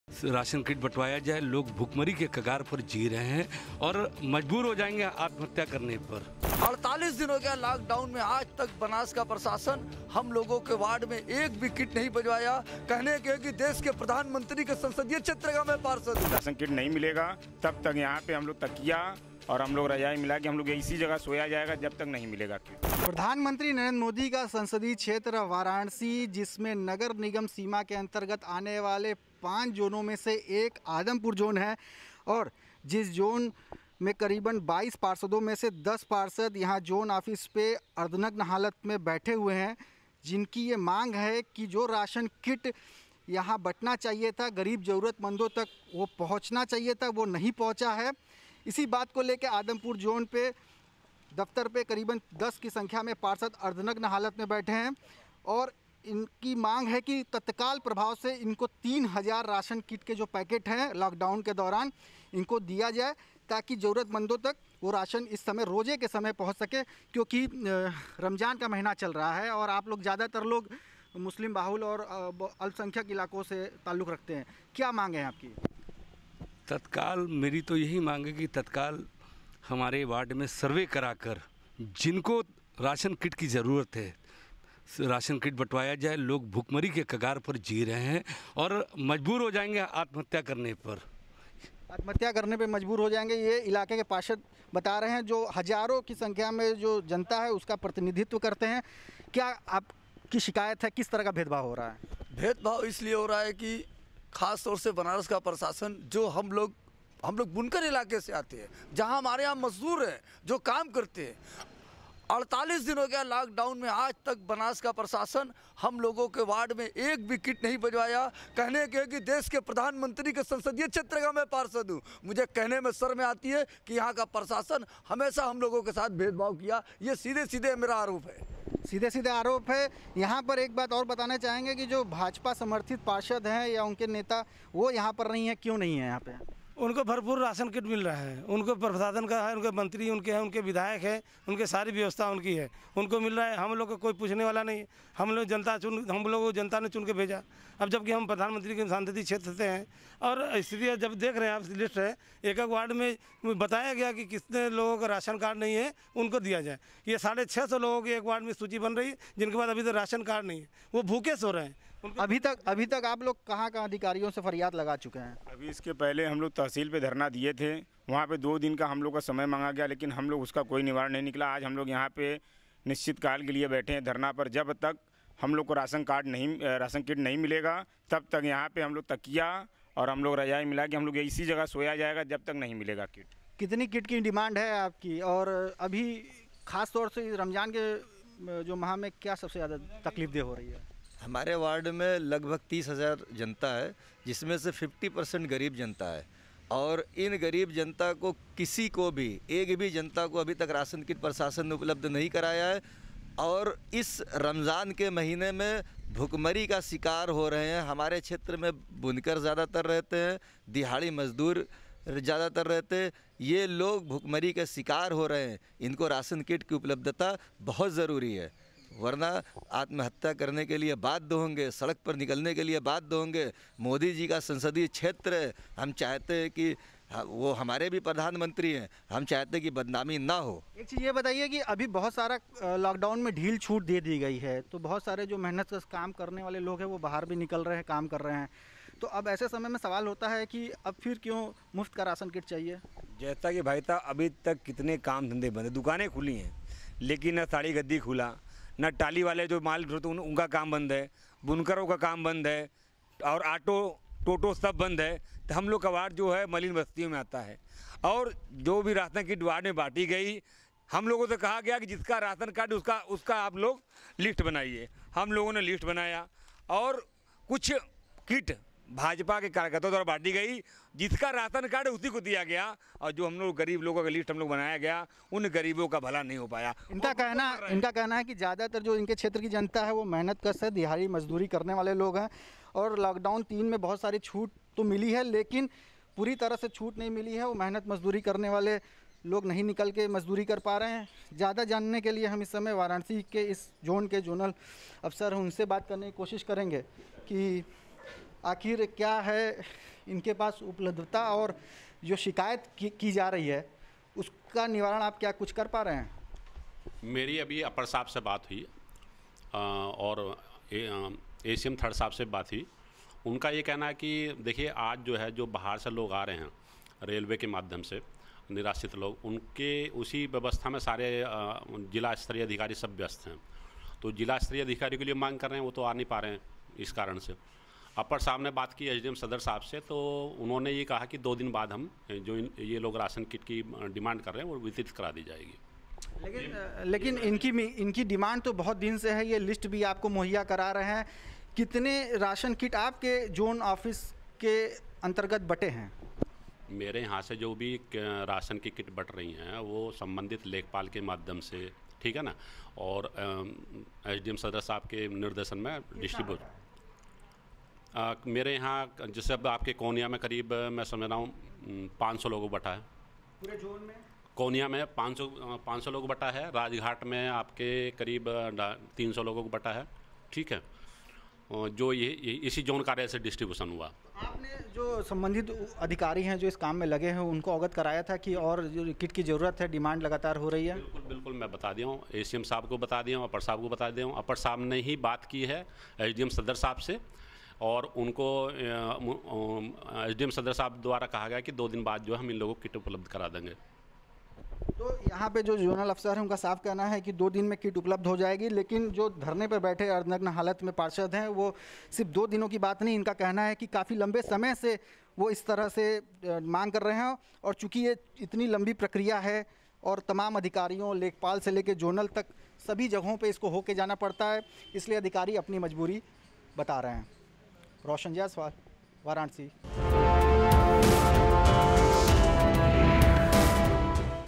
Lockdown के दौरान PM Modi के शहर में नेताओं का धरना प्रदर्शन!
लॉकडाउन के दौरान पीएम मोदी के संसदीय क्षेत्र वाराणसी में प्रशासन के काम से नाराज होकर धरना प्रदर्शन पर बैठ नेता..